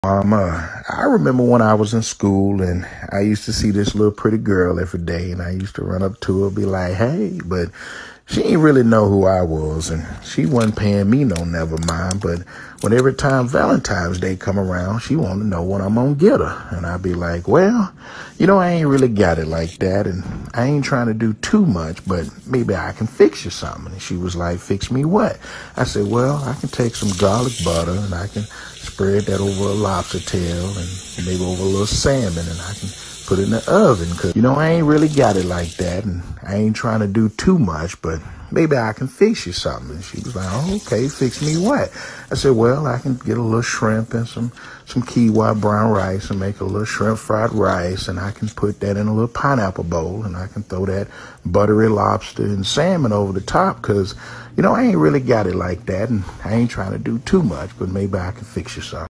Garlic Butter Salmon, Lobster and Shrimp Fried Rice Pineapple Bowl by Pollo Wang
Mammah! I decided to make this pretty girl a ValentinesDay meal Garlic Butter Salmon, Lobster and Shrimp Fried Rice Pineapple Bowl.